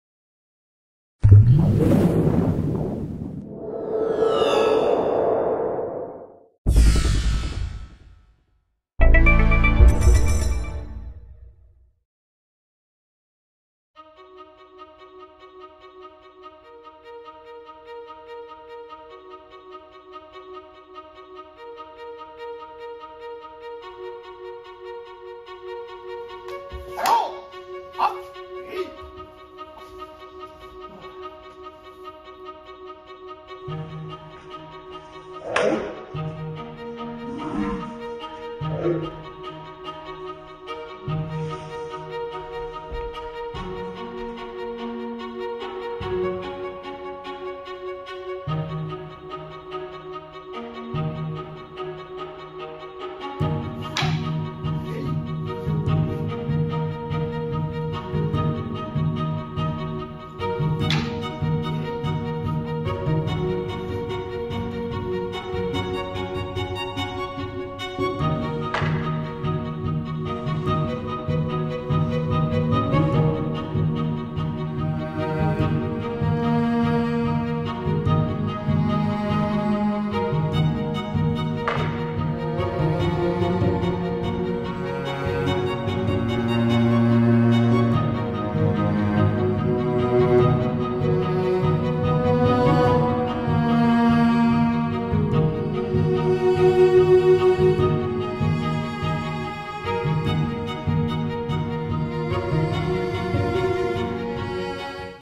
(7*) Ken tai jo shici henka 2 - Marco Rubatto
Variazione del 7º Ken tai jo,  forma di armonizzazione a con proiezione (taijutsu no riai)

7º Ken tai jo variation, harmonization form with throw (taijutsu no riai)